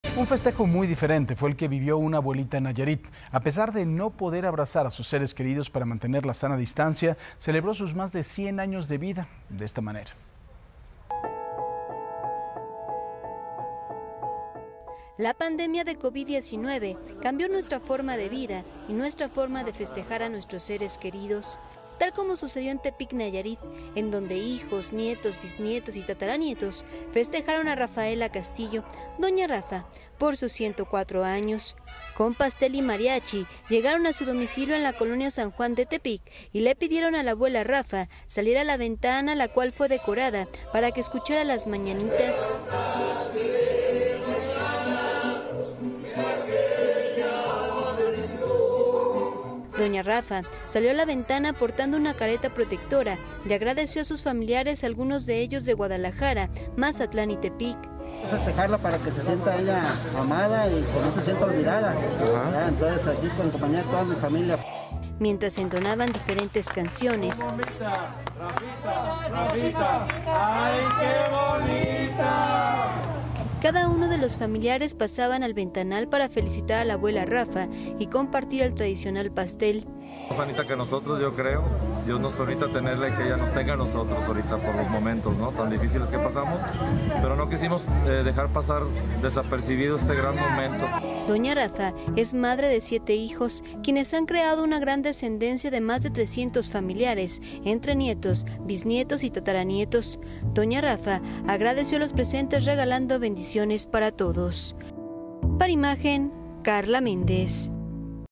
¡Abuelita cumple 104 años y festeja con sana distancia! | Noticias con Francisco Zea
¡La pandemia no la detuvo! Con SanaDistancia, pastel y mariachi, así fue como Doña Rafa, una abuelita de 104 años, festejó su cumpleaños.